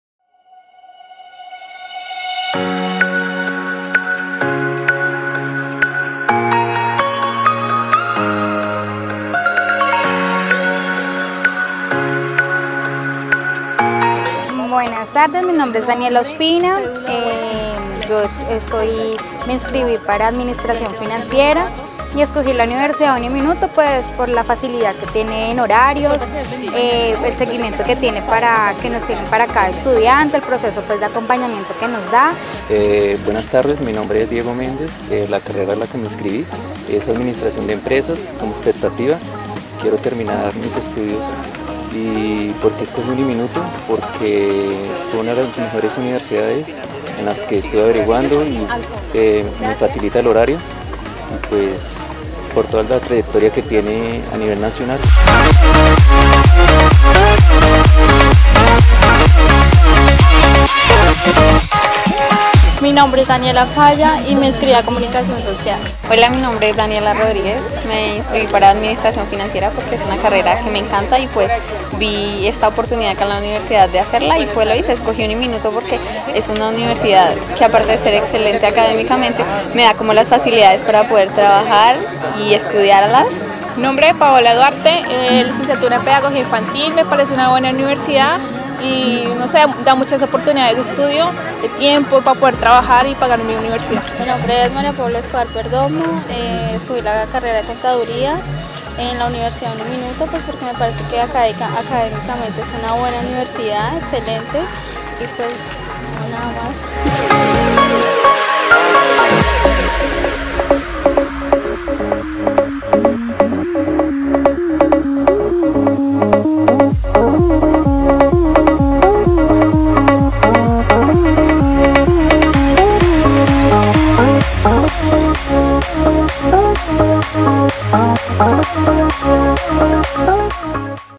Inducción estudiantes nuevos UNIMINUTO Ibagué
Más de ochocientos estudiantes asistieron a las jornadas de inducción que realiza UNIMINUTO tradicionalmente para sus nuevos alumnos.